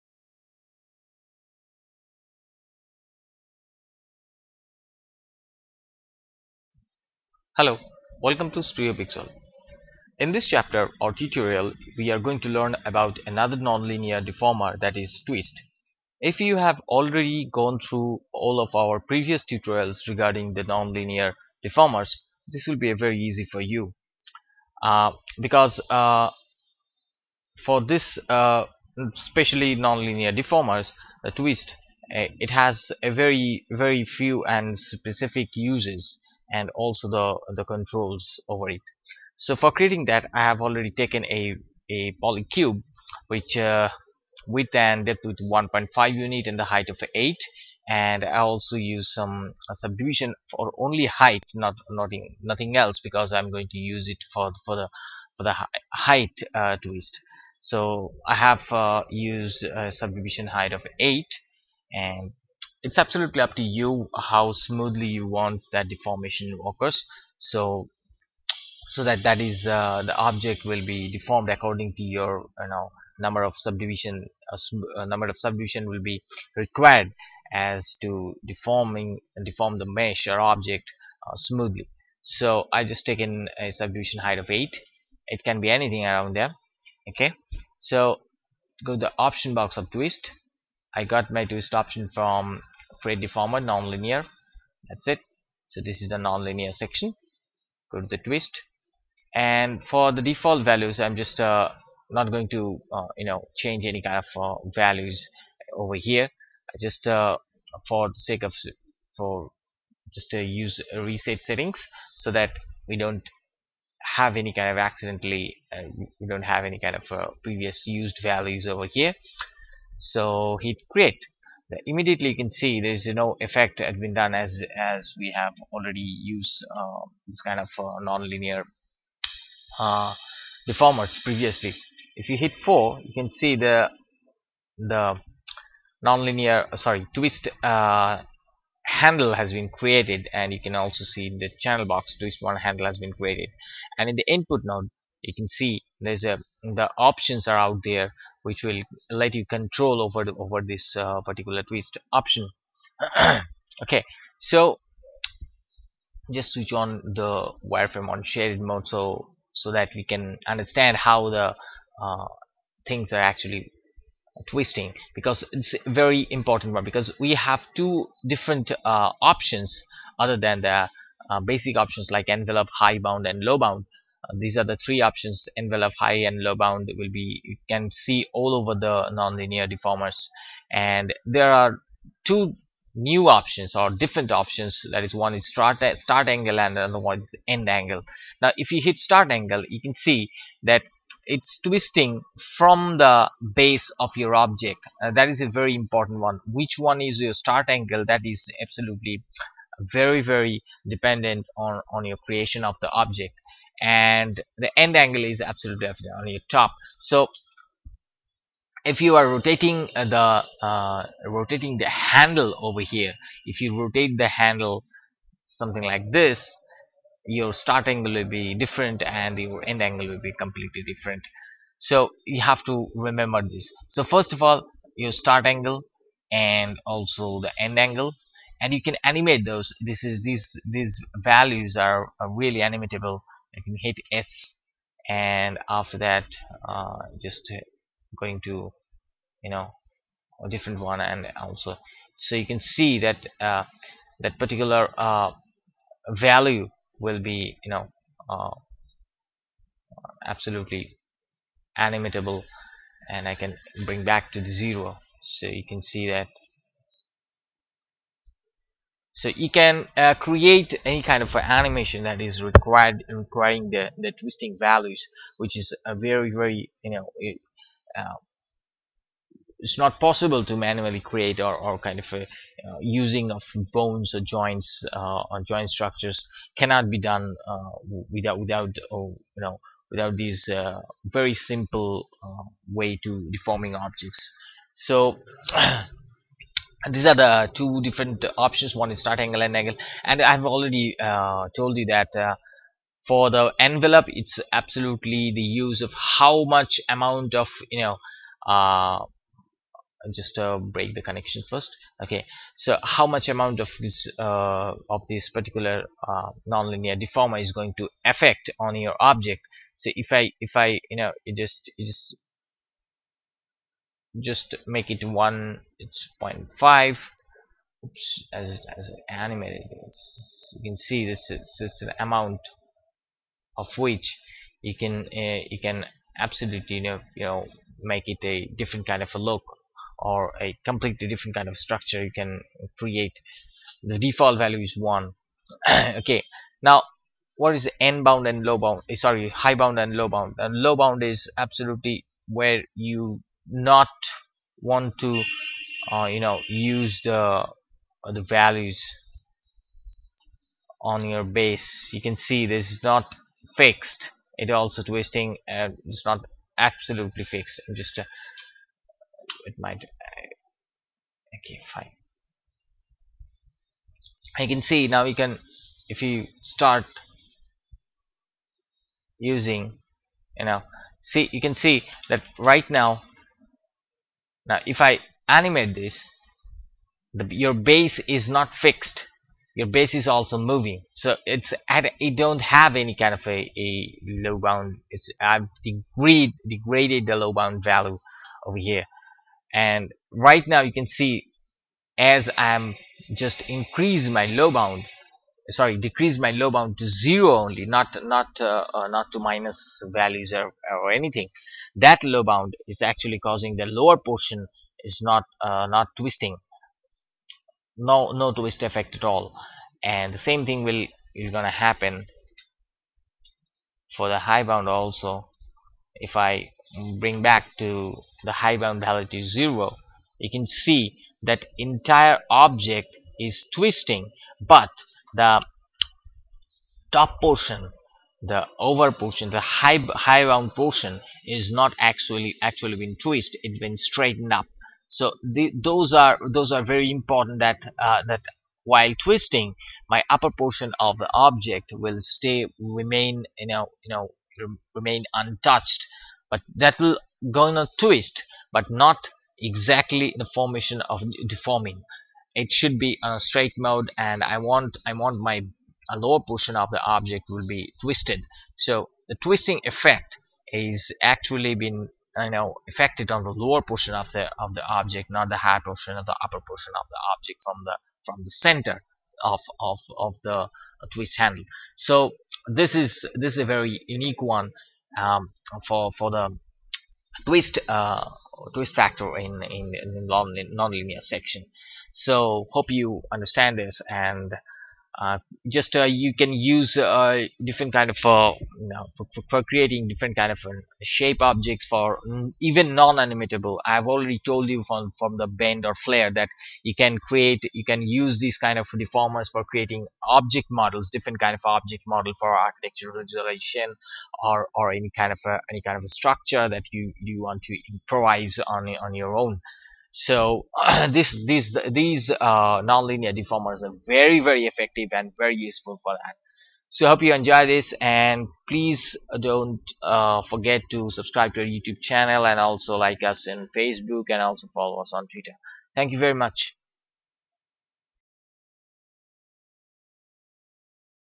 Nonlinear Twist Deformer Tutorial in Autodesk Maya | Maya Basic Tutorial
Twist Deformer, as the name suggests , helps us to create a twist effect on the geometry it applied on.